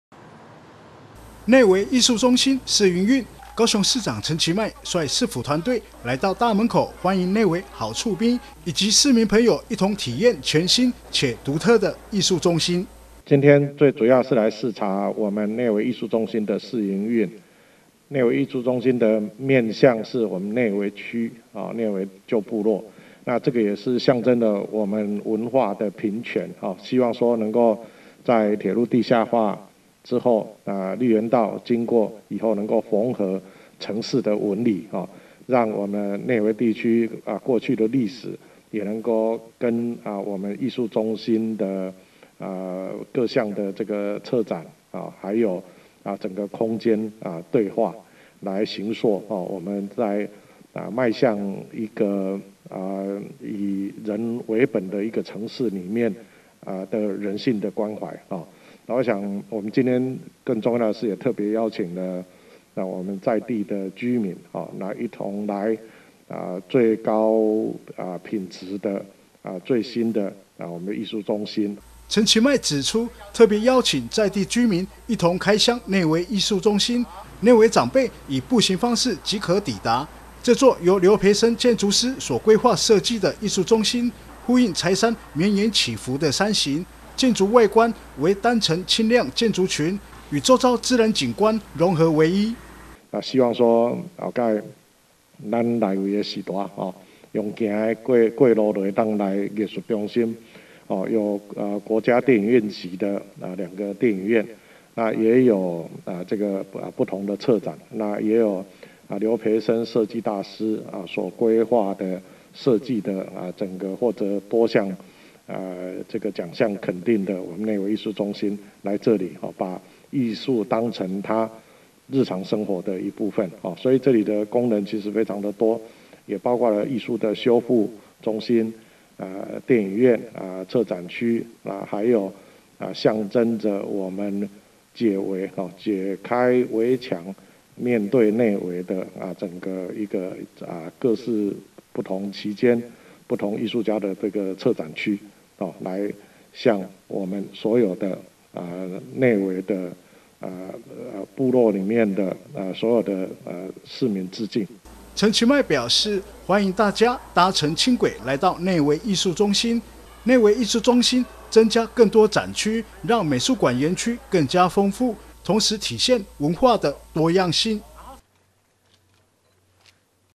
1111101 內惟藝術中心首日試營運 陳其邁：以館名向內惟舊部落致敬
【高雄訊】內惟藝術中心今(1)日試營運，高雄市長陳其邁率市府團隊來到大門口歡迎內惟好厝邊，以及市民朋友一同體驗全新且獨特的藝術中心。陳其邁表示，內惟藝術中心面向內惟舊部落，象徵文化的平權，在鐵路地下化後，綠園道縫合城市紋理，讓內惟地區過去的歷史，能夠與藝術中心各項策展及整個空間對話，形塑高雄以人為本的人性關懷。

陳其邁指出，今天特別邀請在地居民一同開箱內惟藝術中心，內惟長輩以步行方式即可抵達，這座由劉培森建築師所規劃設計的藝術中心，呼應柴山綿延起伏的山形，建築外觀為街廓式單層輕量建築群，與周遭自然景觀融合為一，館內設有兩座國家電影院級的專業影廳、展示場館，也有設計典藏修復展示空間，歡迎民眾到此將藝術當成日常生活的一部分。

陳其邁說，內惟藝術中心首展《解惟．解圍》，象徵「解開圍籬、迎向內惟」的精神，並提及，當時在命名館名時，特別取名為內惟，意在向內惟居民致敬，而館內動線設計也是直接面向內惟，而非原先設計的面向美術館園區；他再次歡迎大家搭乘輕軌來到內惟藝術中心，內惟藝術中心增加更多展區，讓美術館園區更加豐富，同時體現文化的多樣性。

文化局表示，除了首展《解惟．解圍》，亦展出由泰國印度裔藝術家阿運．若望恰庫（Navin Rawanchaikul）所創作的《內惟戲院》系列，透過社區訪談以及文史脈絡爬梳，折射出台灣與泰國共同的歷史與身分認同議題，今日策展團隊也邀請作品內的「畫中人」內惟居民們共同前來一起欣賞阿運的創作。

內惟藝術中心即日起試營運，搭乘高雄輕軌至內惟藝術中心站下車步行1分鐘即可抵達，館內結合當代展覽、主題影展、修復中心、設計選物以及咖啡輕食等服務，試營運期間為維護與測試觀展品質，採分時分流入館機制，系統保障半數名額提供內惟居民預約，凡有高雄內惟居民身分者，預約即享專屬好康加碼贈「內惟戲院」電影兌換劵一張，歡迎至官網（ ）預約。